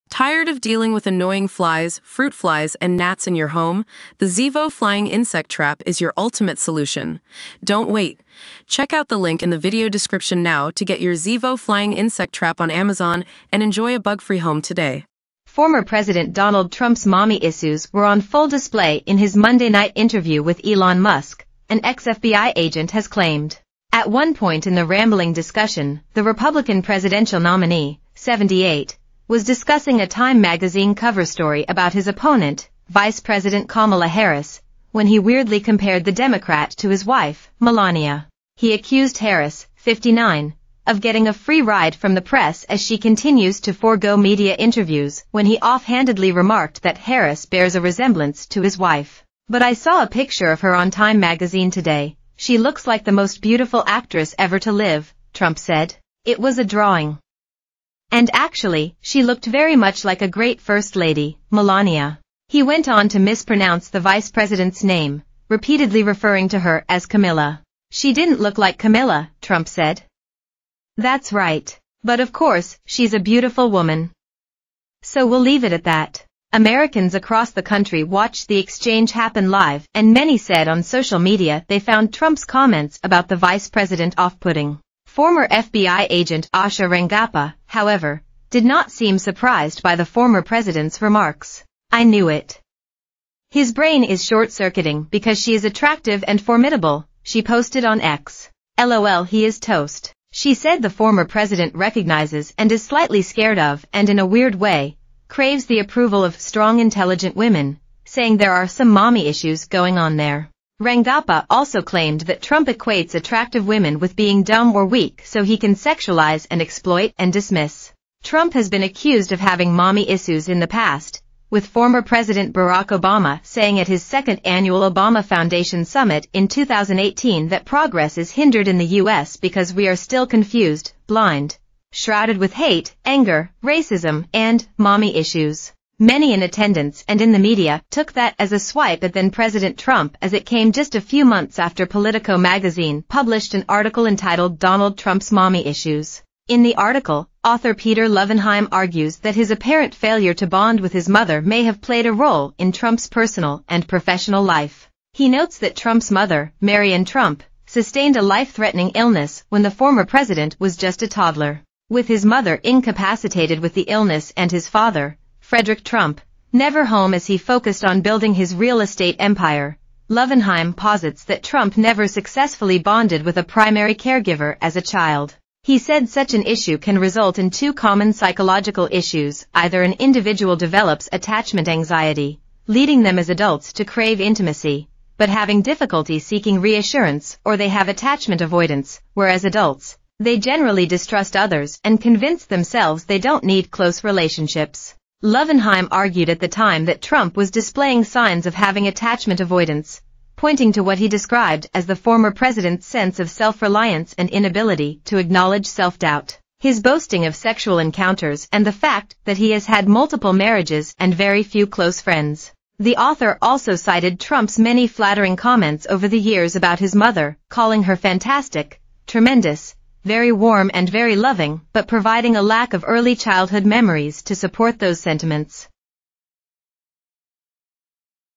Trump's 'mommy issues' came to light in Musk interview, ex-FBI agent
Trump's 'mommy issues' came to light in Musk interview, ex-FBI agent says